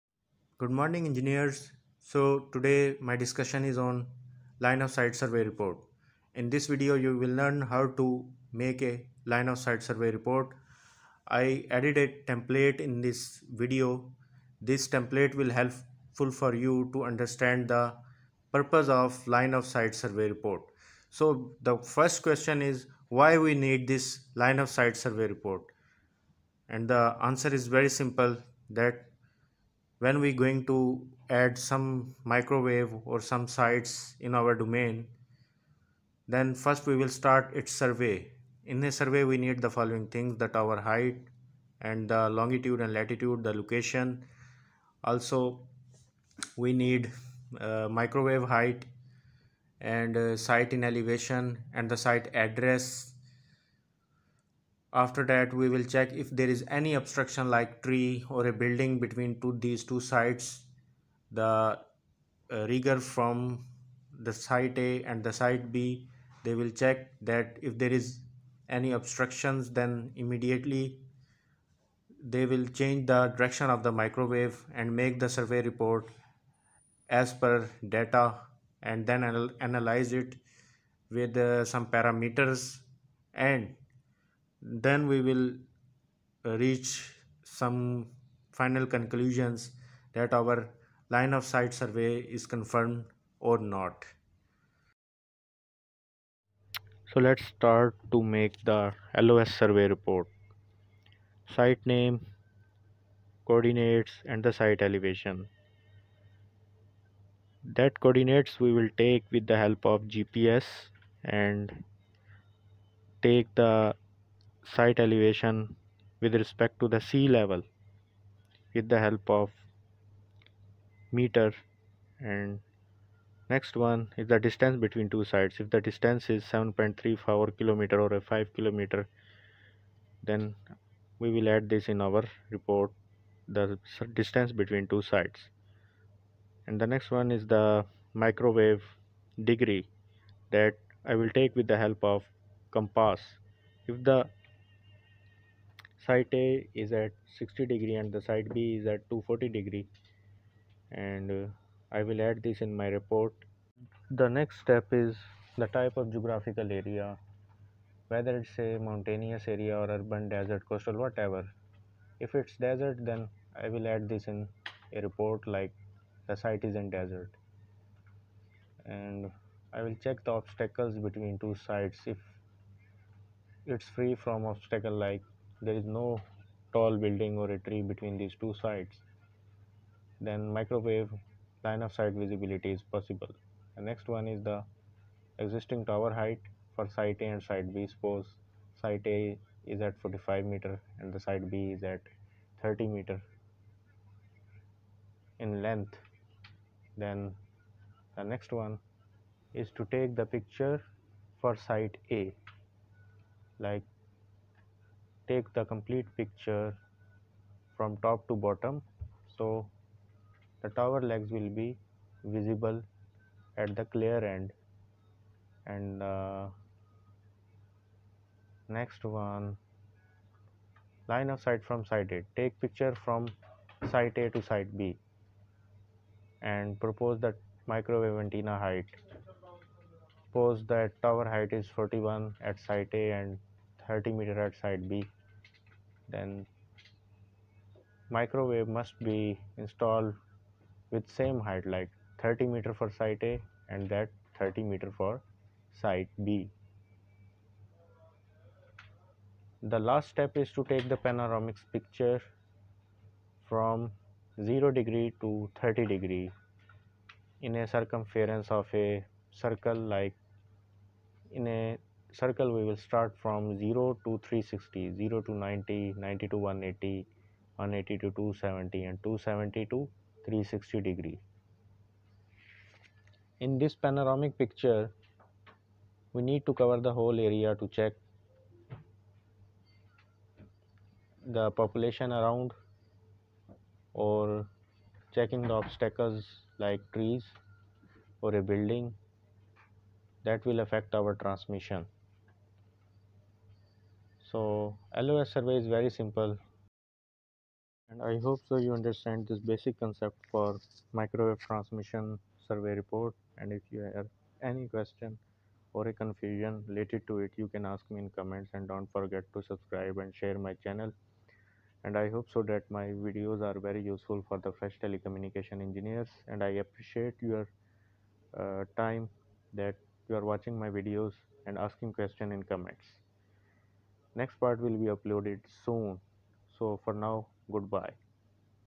Microwave LOS Survey Report||What is Microwave Transmission Survey
Microwave transmission survey is used to check the Line of sight visibility between two microwaves end and it involves the following steps .(1) check tower height (2) Visibility of microwaves between both ends .(3) check the obstacles like tall buildings and trees (4) Check the Azimuth for Both ends (microwave must be at same height and must be in front of each other).

transmission survey
Line of sights Survey
Microwave transmission.

We do not intend to reduce any views from the original content but my aim is to educate  people through my technical videos .